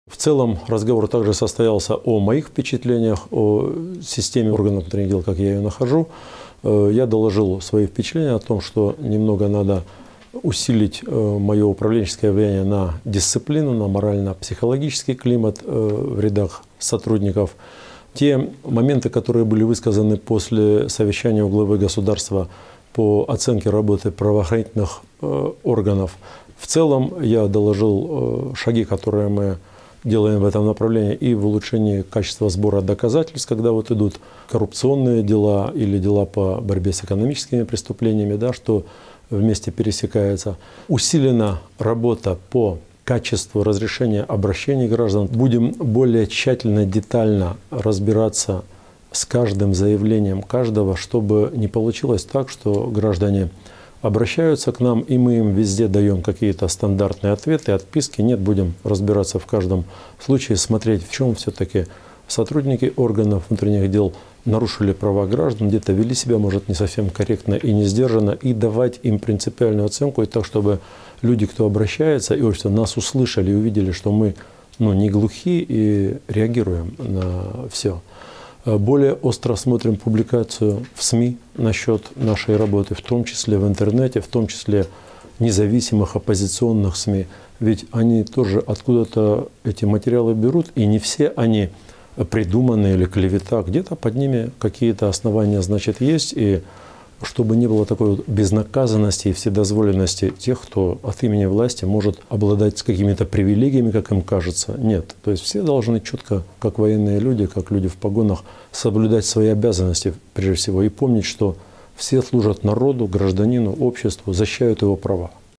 МВД Беларуси усиливает работу с обращениями граждан и реагирование на публикации в СМИ
Министерство внутренних дел Беларуси усиливает работу с обращениями граждан и реагирование на критические публикации в СМИ. Об этом сегодня заявил журналистам министр внутренних дел Беларуси Юрий Караев после своего доклада главе государства.